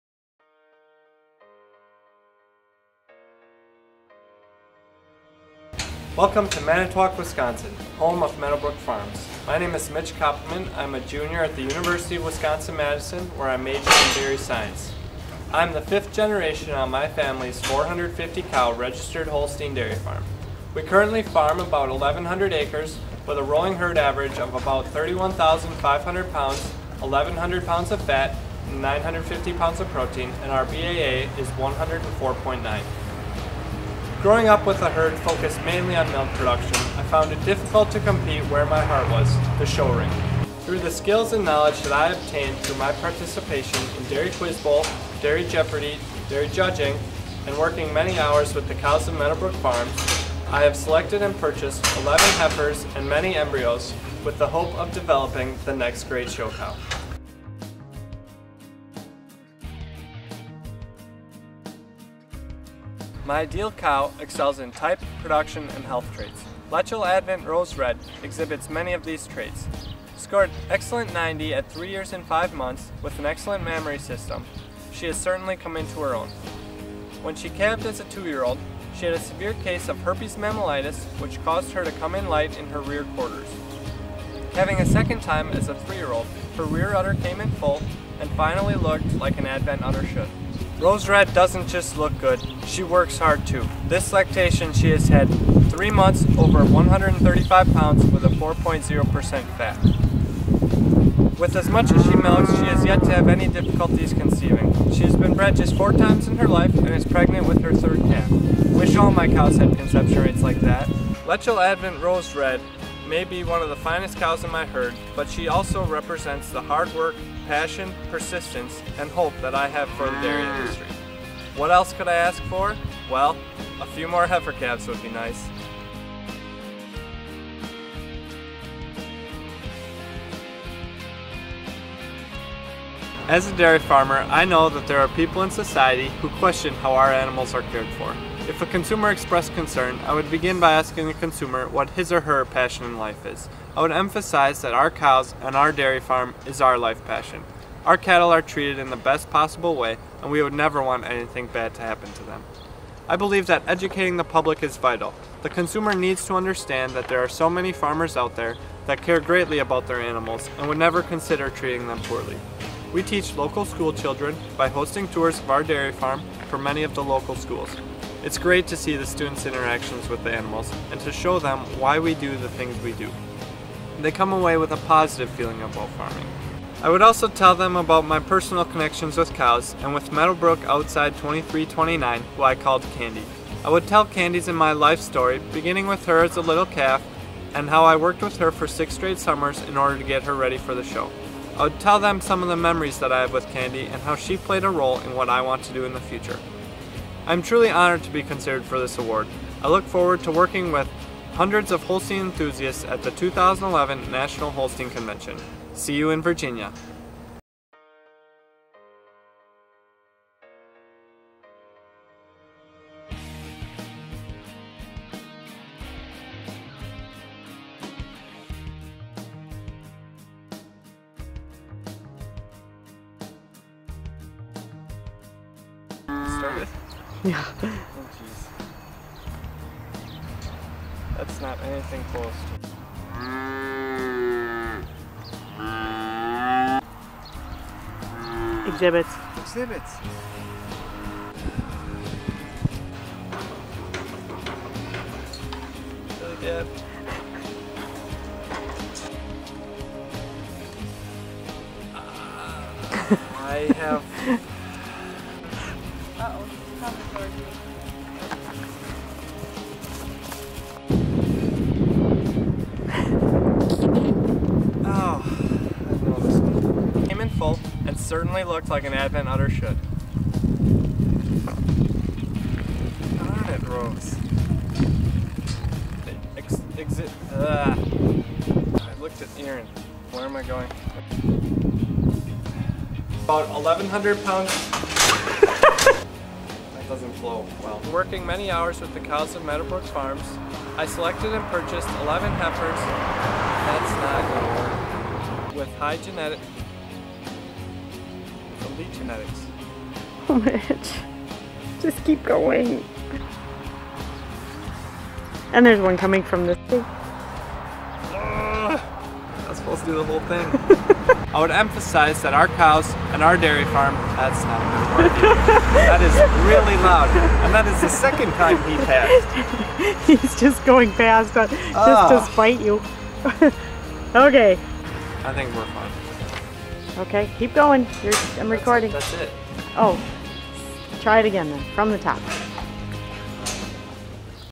Mitchell Kappelman - 2011 Holstein USA Distinguished Junior Member Finalist
This year's Distinguished Junior Member semifinalists were asked to submit a video giving an introduction of themselves and their farm, and then answer two personal view questions (their choice out of four). Though the videos weren't judged as a part of their final score for the award, we think they did an awesome job and wanted to showcase their hard work! The top six finalists were named during the National Junior Holstein Convention in Richmond, Va., on June 25, 2011.